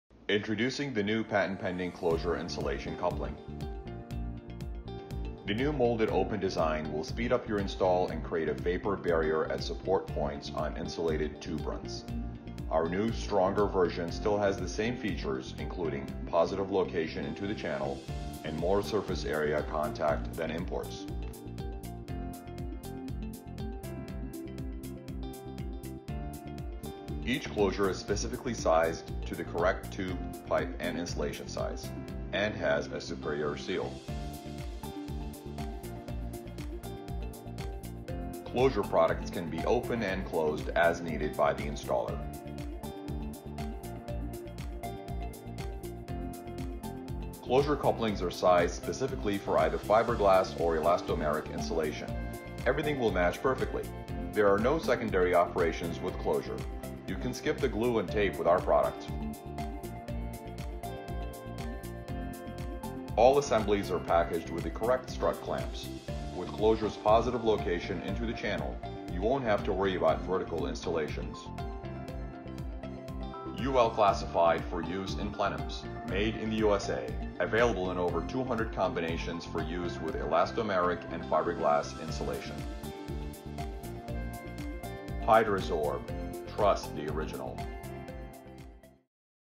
Patented Klo-Shure Insulation Couplings
The new molded open design will speed up your install and create a vapor barrier on support points on insulated tube runs.  Klo-Shure has more surface area contact than imports. Klo-Shure products can be open and closed as needed by the installer. Klo-Shure couplings are sized specifically for elastomeric or fiberglass installation, and available in over 200 combinations.